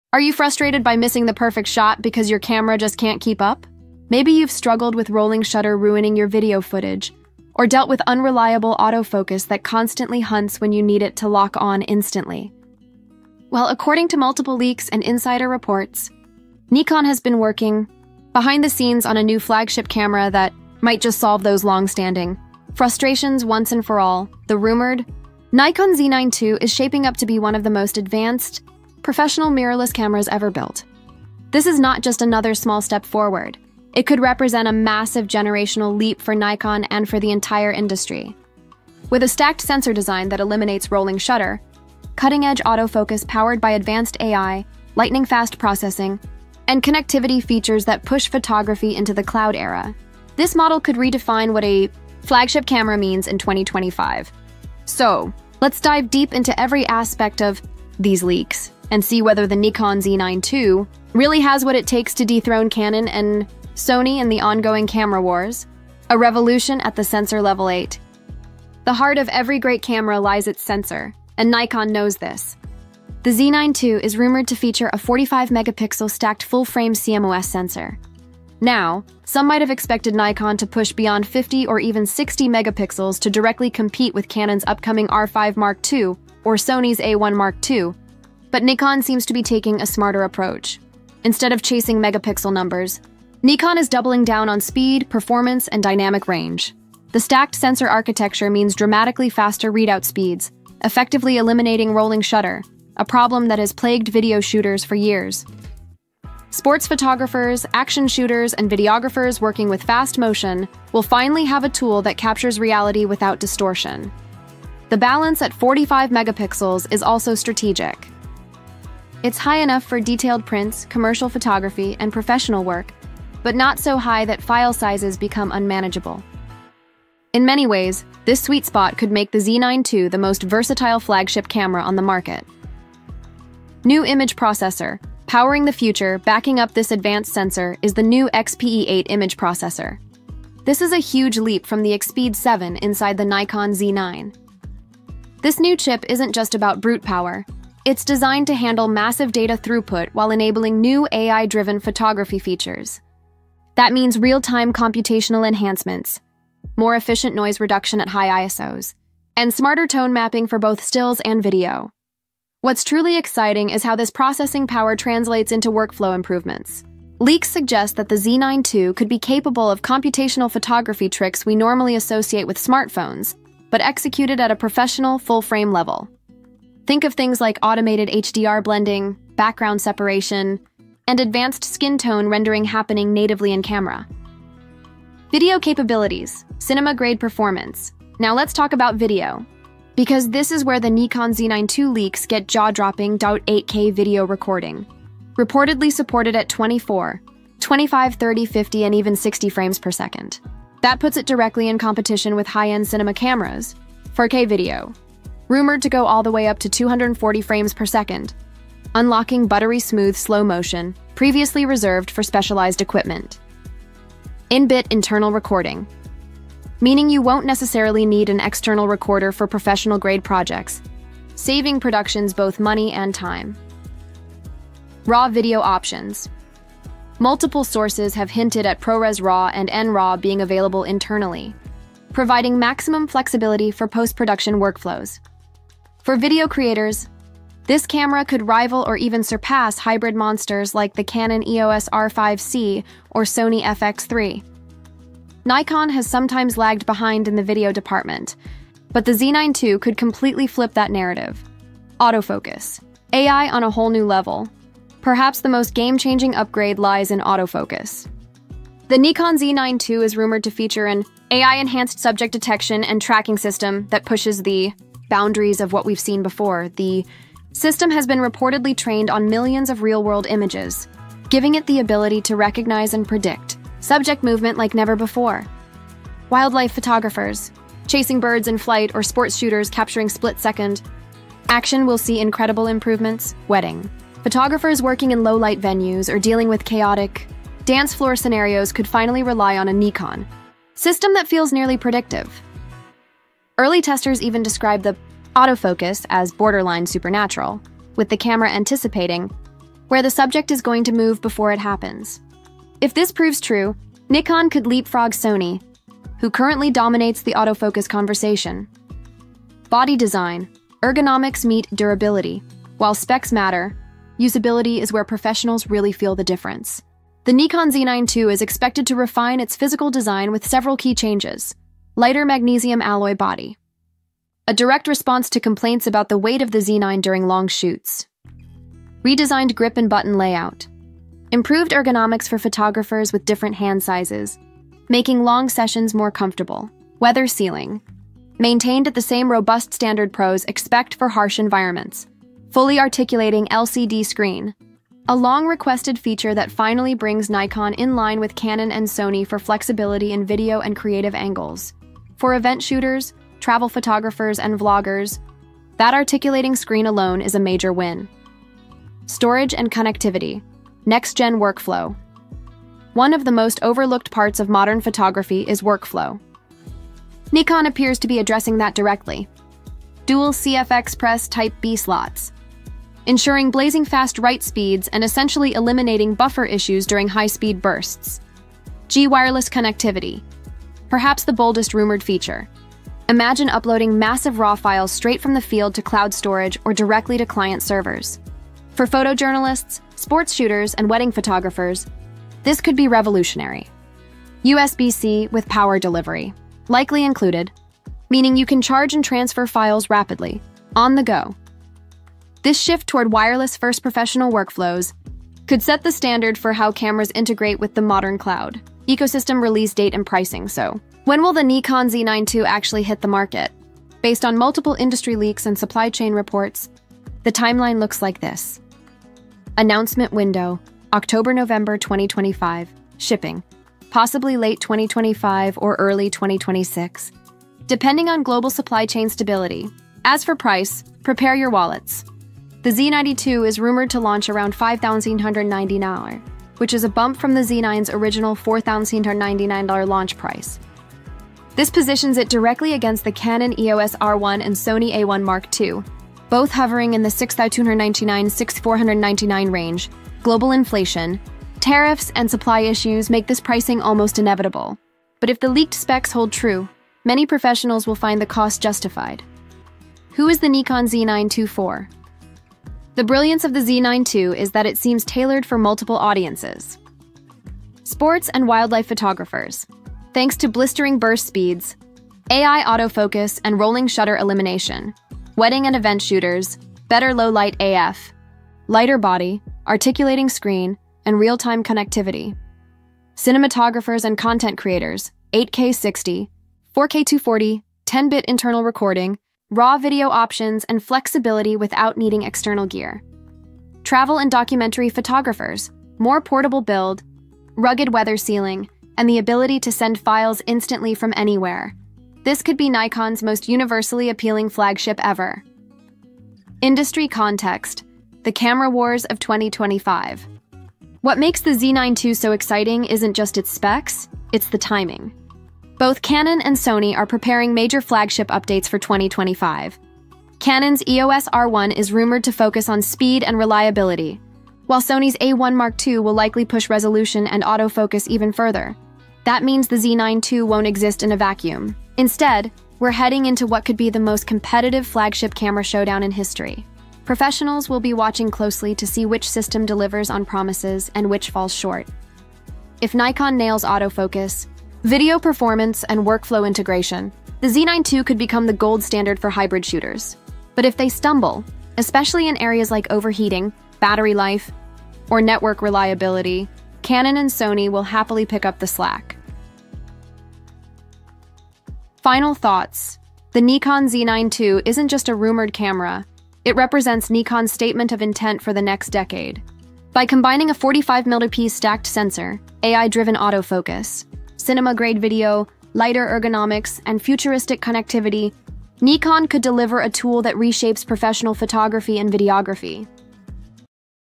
Nikon Z9 Mark II – Unbelievable Specs, Release Date Revealed & Price!
While no official pricing has been leaked, a price increase over the original Z9 is highly likely. Given the rumors of significant performance and feature upgrades, the Nikon Z9 Mark II is expected to be positioned at a premium price point, possibly exceeding the launch price of the original Z9 (which was around $5,500 USD).